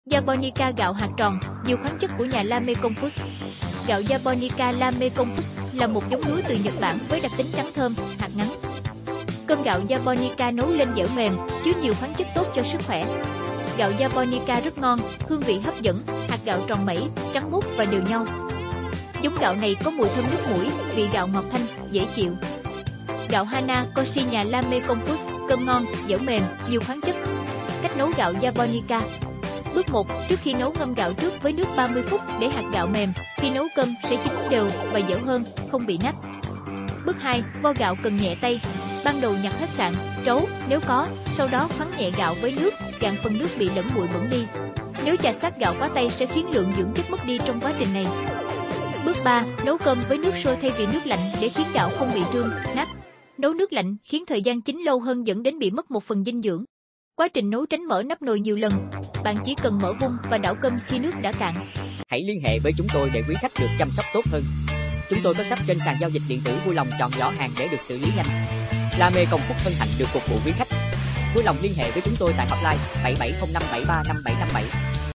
La Mekong Foods || 🌾🍘🍙🍱🍥🥗☘️Japonica gạo hạt tròn La Mekong Foods 🌾🍘🍙🍱🍥🥗☘️
🌾🍘🍙🍱🍥🥗☘️Japonica gạo hạt tròn La Mekong Foods 🌾🍘🍙🍱🍥🥗☘️

🥗☘️ Gạo Japonica là một giống lúa từ Nhật Bản với đặc tính trắng thơm, hạt ngắn.
🥗☘️ Cơm gạo Japonica nấu lên dẻo mềm, chứa nhiều khoáng chất tốt cho sức khỏe
🥗☘️ Gạo Japonica rất ngon, hương vị hấp dẫn, hạt gạo tròn mẩy, trắng muốt và đều nhau.
🥗☘️Giống gạo này có mùi thơm nức mũi, vị gạo ngọt thanh, dễ chịu.
Hướng dẫn nấu:
🍥🥗Cách nấu gạo
Japonica🍥🥗
🌾Bước 1: Trước khi nấu ngâm gạo trước với nước 30 phút để hạt gạo mềm, khi nấu cơm sẽ chín đều và dẻo hơn, không bị nát.
🌾Bước 2: Vo gạo cần nhẹ tay. Ban đầu nhặt hết sạn, trấu (nếu có), sau đó khoắng nhẹ gạo với nước, gạn phần nước bị lẫn bụi bẩn đi. Nếu chà xát gạo quá tay sẽ khiến lượng dưỡng chất mất đi trong quá trình này.
🌾Bước 3: Nấu cơm với nước sôi thay vì nước lạnh để khiến gạo không bị trương, nát. Nấu nước lạnh khiến thời gian chín lâu hơn dẫn đến bị mất một phần dinh dưỡng. Quá trình nấu tránh mở nắp nồi nhiều lần. Bạn chỉ cần mở vung và đảo cơm khi nước đã cạn.